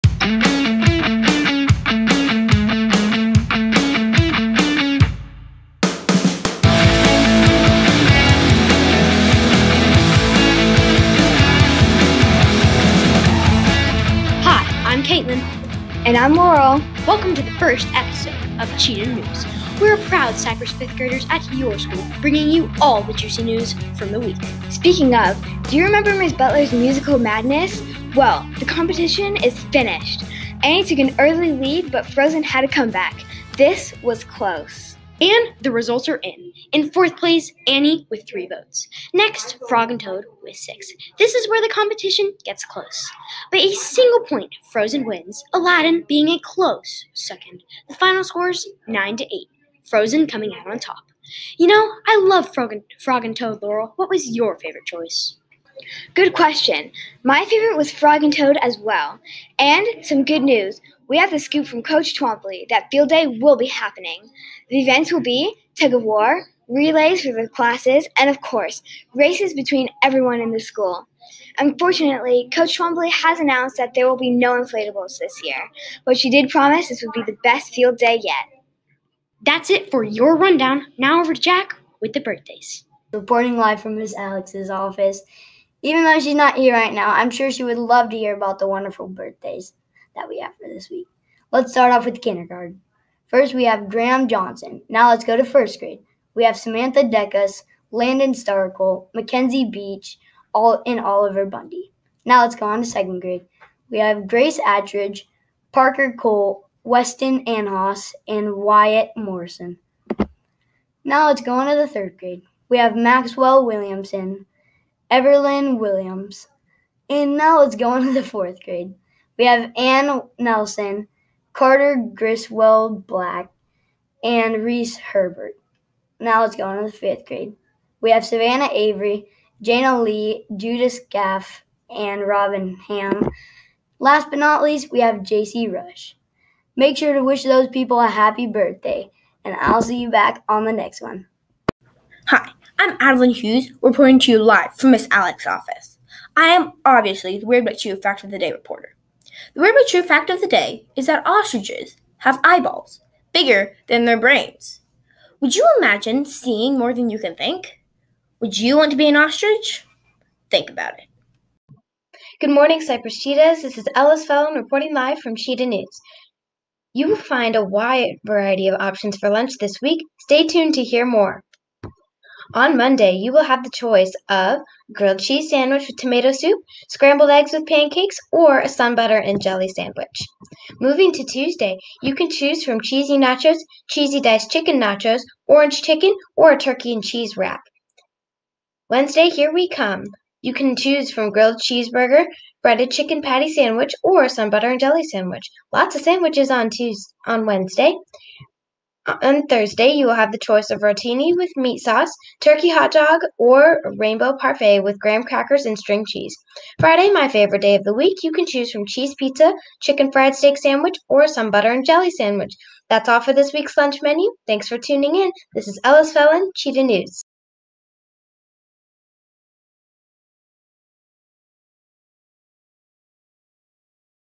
2021.04.16 Cheetah News Episode 1
Episode 1 of the Cypress Elementary Student Newscast for Friday, April 16, 2021.